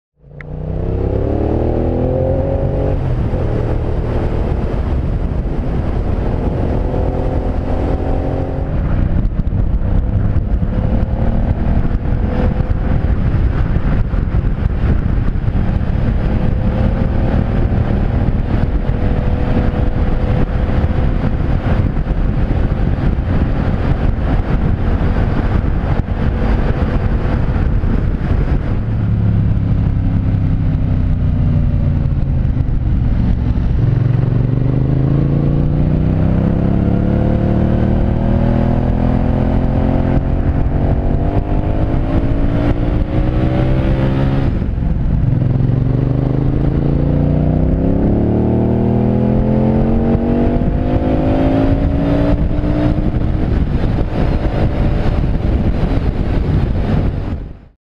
Aprilia Caponord ride in Felixstowe, Suffolk
Getting to grips with the Aprilia Caponord, and trying out a new camera position looking back at the rider. Exhaust sound is good, thanks to the butterfly valve being modified to be always open.

2013 Ducati Multistrada keeping pace behind me.